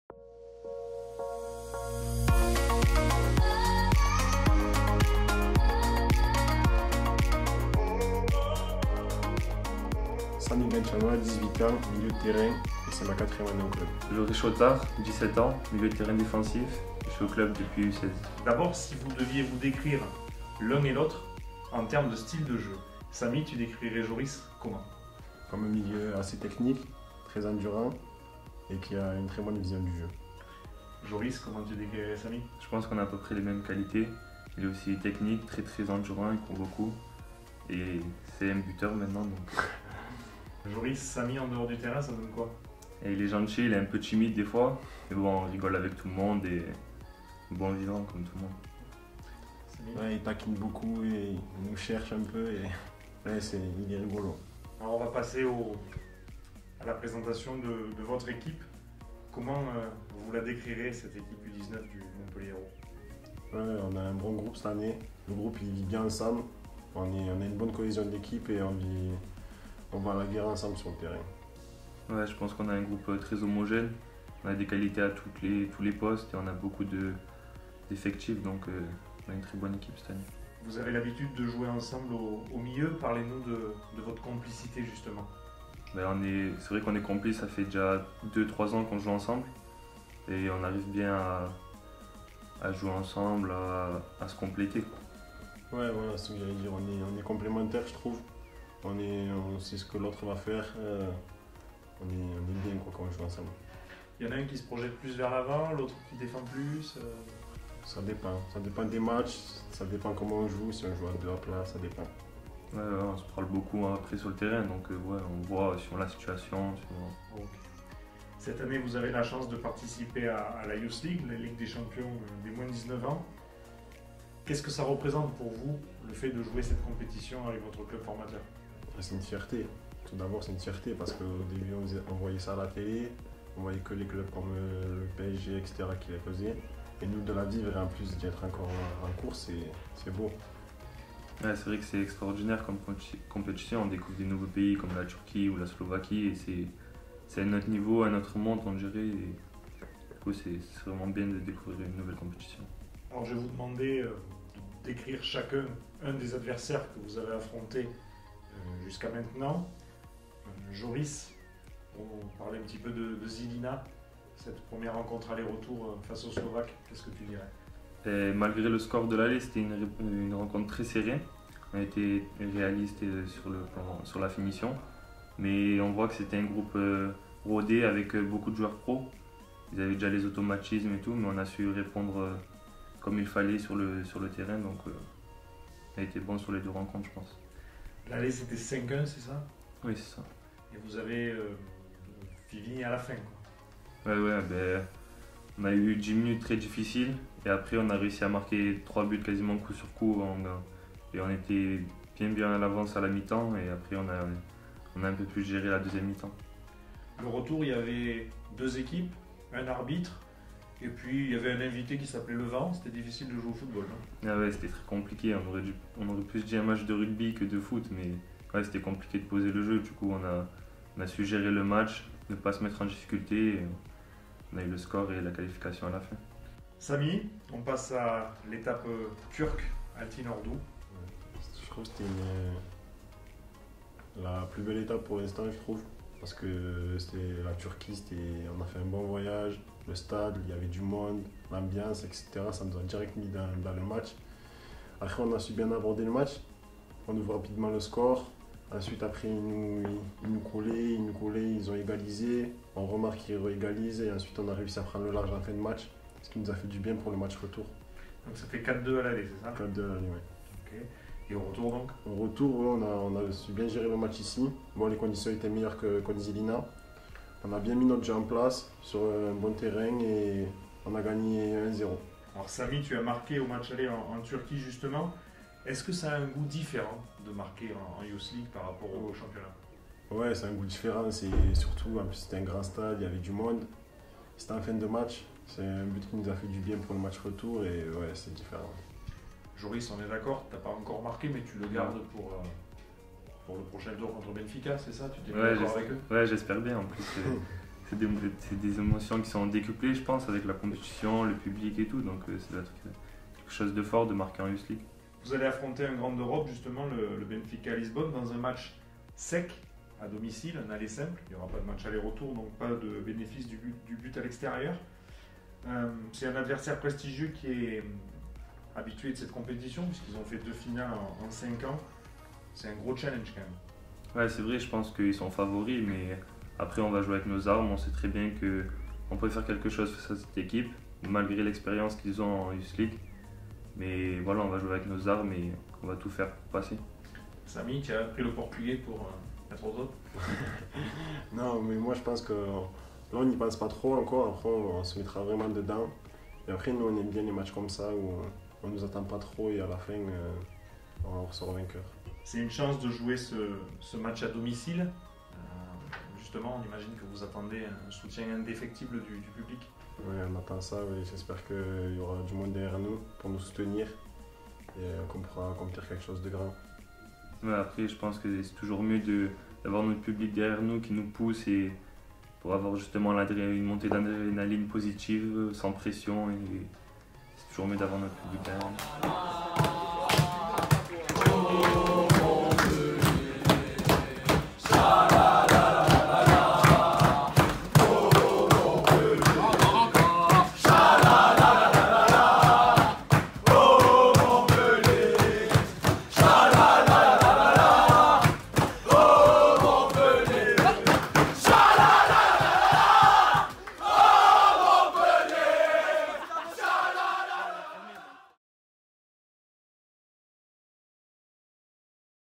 Á la rencontre de Joris Chotard et Samy Benchama !
A la rencontre de Joris Chotard et Samy Benchama avant d’affronter le Benfica Lisbonne en UEFAYouthLeague 🏆
💬 « C’est une compétition extraordinaire… »